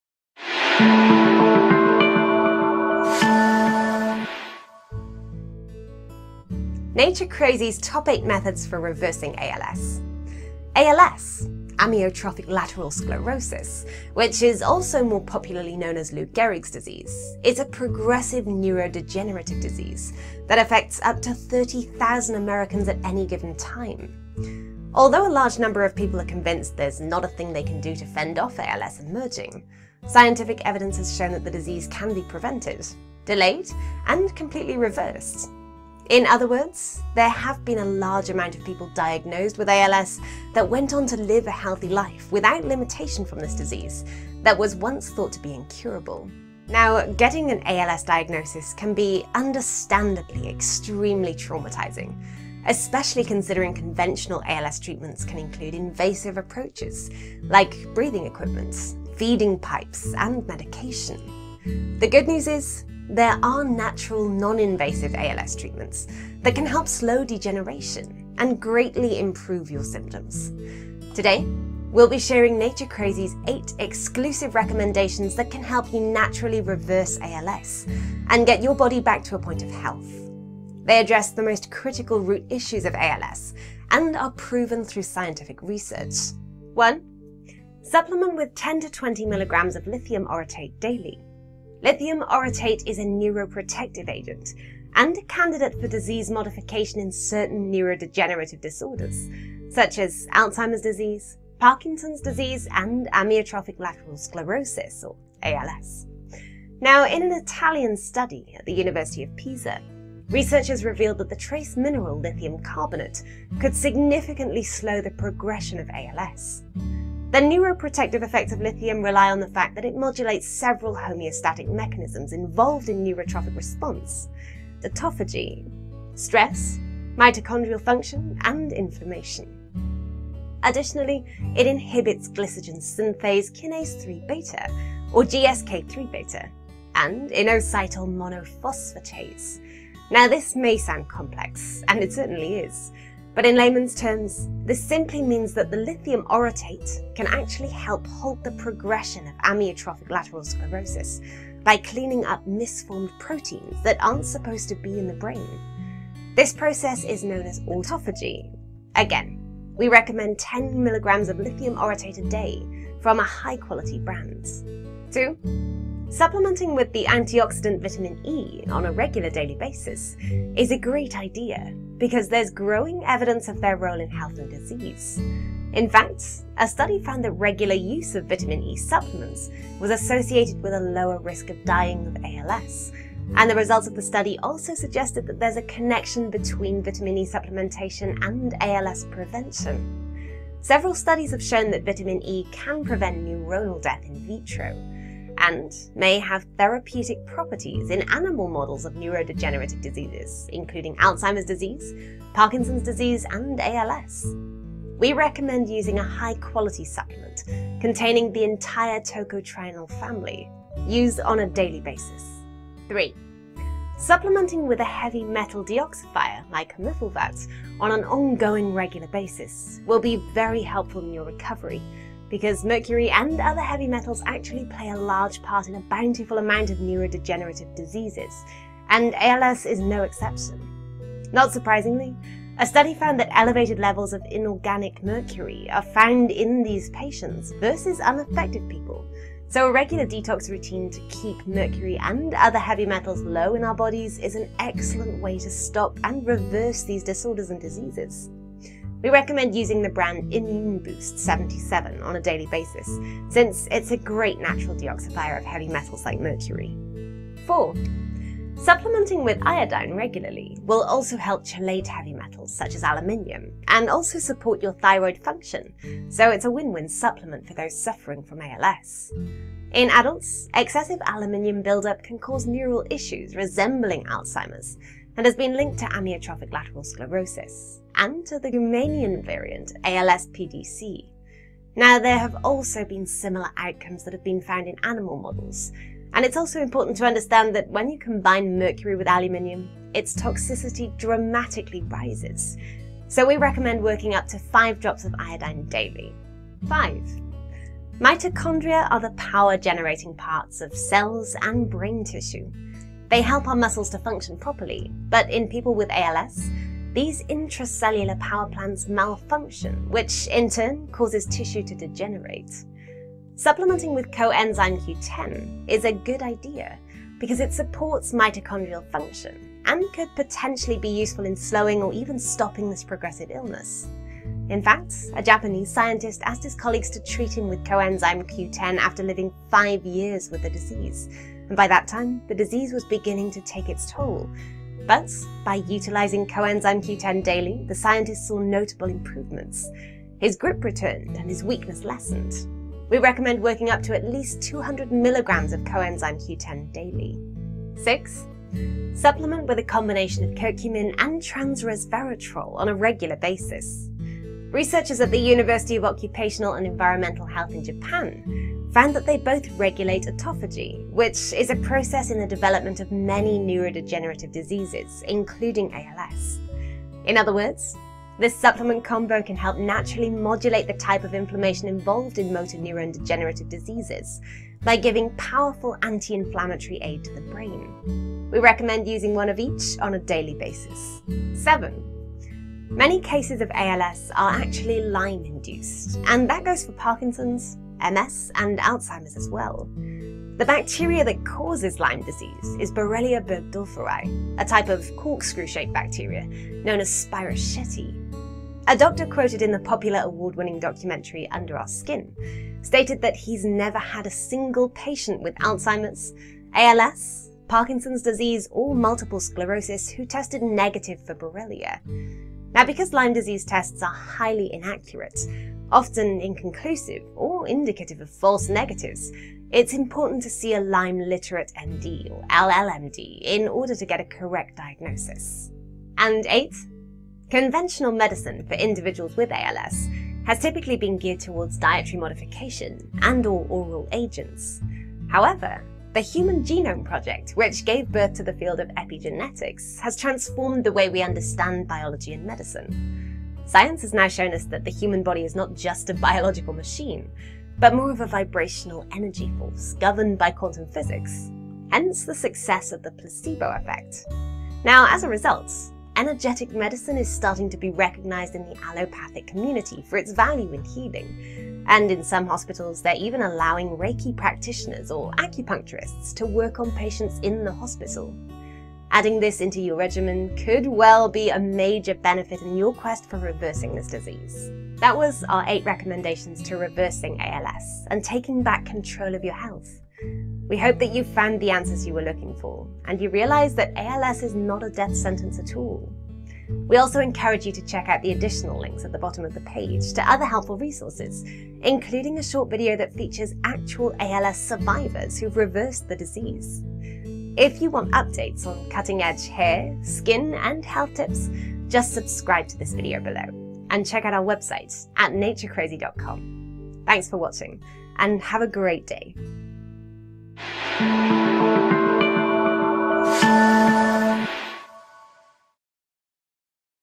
Nature Crazy's Top 8 Tips For Reversing ALS / Lou Gehrig’s Disease
Emerging scientific evidence has shown that ALS can be prevented, delayed, and completely reversed.

In today's video we'll be sharing Nature Crazy's 8 exclusive recommendations that can help you naturally reverse ALS and get your body back to a point of health.

1. Supplement with 10 to 20 mg of Lithium Orotate daily. An Italian study at the University of Pisa revealed that the trace mineral lithium carbonate could significantly slow the progression of ALS.  The neuroprotective effects of lithium rely on the fact that it modulates several homeostatic mechanisms involved in neurotrophic response and autophagy.

2. Supplementing with the antioxidant Vitamin E on a regular daily basis is a great idea because there is growing evidence of its role in health and disease. A study found that regular use of vitamin E supplements was associated with a lower risk of dying of ALS and the results of the study also suggested there is a connection between vitamin E supplementation and ALS prevention.

3. Supplementing with a heavy metal detoxifier like Humifulvate on an ongoing regular basis will be very helpful in your recovery because mercury and other heavy metals actually play a large part in a bountiful amount of neurodegenerative diseases and ALS is no exception. a study found that elevated levels of inorganic mercury are found in these patients versus unaffected people so a regular detox routine to keep mercury and other heavy metals low in our bodies is an excellent way to stop and reverse these disorders and diseases.

4. Supplementing with Iodine regularly will also help chelate heavy metals such as aluminum and also support your thyroid function so it’s a win win supplement for those suffering from ALS. In adults, excessive aluminum build up can cause neural issues resembling Alzheimer’s, and has been linked to Amyotrophic Lateral Sclerosis and to the Guamanian variant, ALS-PDC. We recommned working up to 5 drops daily.

5. Mitochondria are the power generating parts of all cells and brain tissue. They help our muscles to function properly but in people with ALS these intracellular power plants malfunction which in turn causes the cells and nerve tissue to degenerate. Supplementing with Co-enzyme Q10 is a good idea because it supports mitochondrial function and could potentially be useful in slowing or even stopping this progressive illness.

6. Supplement with a combination of Curcumin and Trans-resveratrol on a regular basis. Researchers at the University of Occupational and Environmental Health in Japan found they both regulate autophagy, which is a process in the development of many neurodegenerative diseases including ALS. In other words, this supplement combo can help naturally modulate the type of inflammation involved in motor neuron degenerative diseases by giving powerful anti-inflammatory aid to the brain. We recommend using one of each on a daily basis.

7. Many cases of ALS are actually lyme induced and that goes for Parkinson's, MS and Alzheimer's as well. Because Lyme disease tests are highly inaccurate, often inconclusive or indicative of false negatives it’s important to see a “Lyme Literate MD (LLMD)” in order to get a correct diagnosis.

8. The field of epigenetics has transformed the way we understand biology and medicine. Science has now shown us that the human body is not just a biological machine but more of a vibrational energy force, governed by quantum physics, hence the success of the placebo effect. As a result, energetic medicine is starting to be recognized in the Allopathic community for its value in healing and even some hospitals are now allowing Reiki practitioners or Acupuncturists to work on patients in the hospital. Adding this into your regimen as well could be of major benefit.

Links:
1. This video substantiates long standing survivors of ALS.
2. An additional resource of information.
3. Another good source of valuble information.